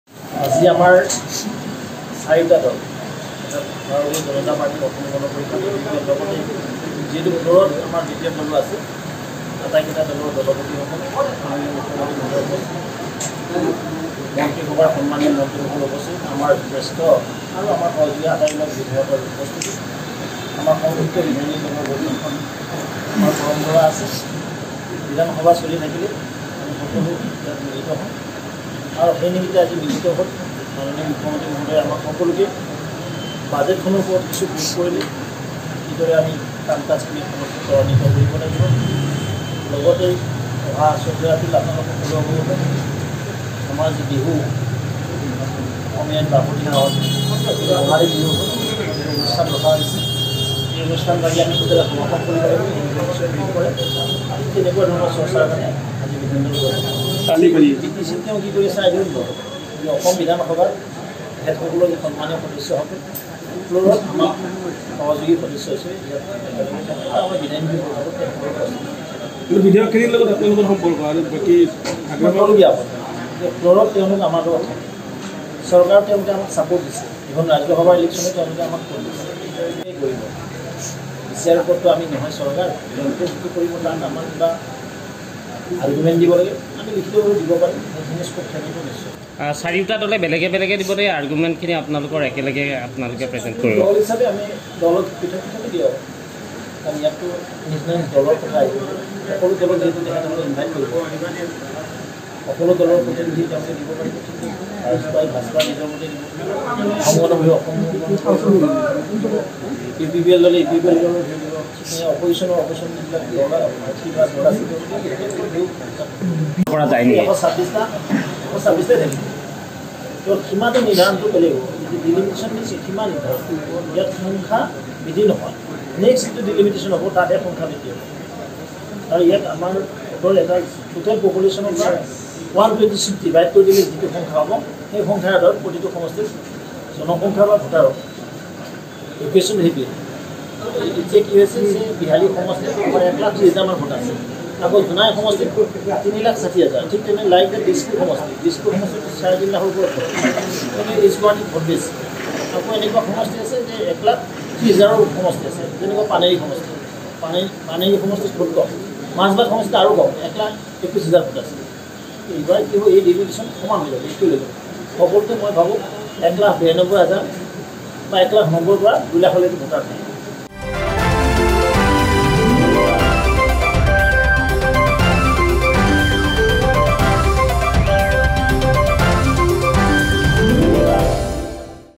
সমষ্টি পুন:নিৰ্ধাৰণক লৈ বিজেপি মিত্ৰজোঁটৰ বৈঠক
মুখ্যমন্ত্ৰীৰ উপস্থিতিত বিধানসভা প্ৰেক্ষাগৃহত এই বৈঠক